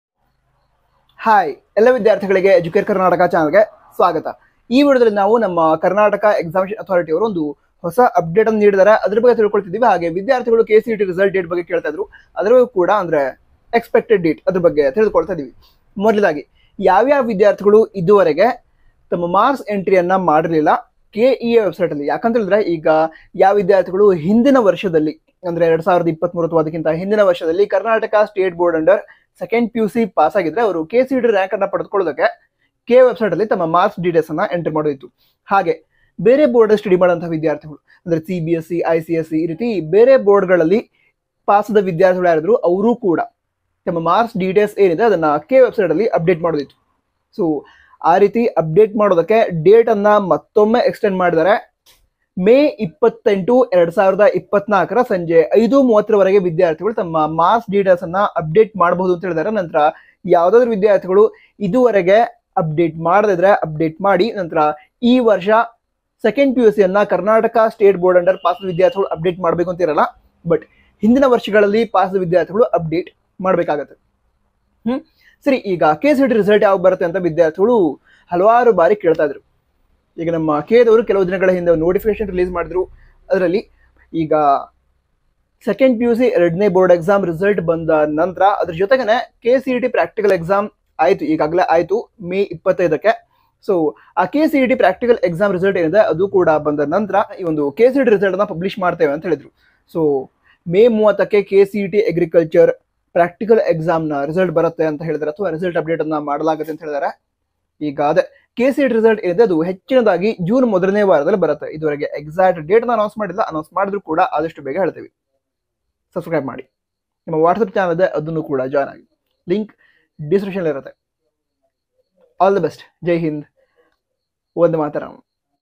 GOOD NEWS FOR KCET 2024 STUDENTS FROM KEA | KCET RESULT 2024?
In this video we are giving information about latest update from Karnataka examinations authority regarding marks entry by students to get the kcet rank for the year 2024.